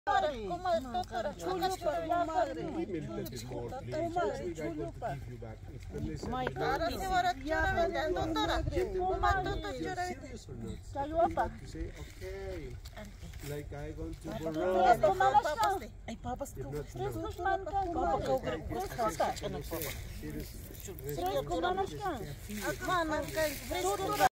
2024 03 21 Peru - Puno - Lago Titicaca - Ilha de Nativos 083510
Adelino's Films - AF
Peru - Puno - Lago Titicaca - Visita a uma ilha de nativos, em 21 de março de 2024.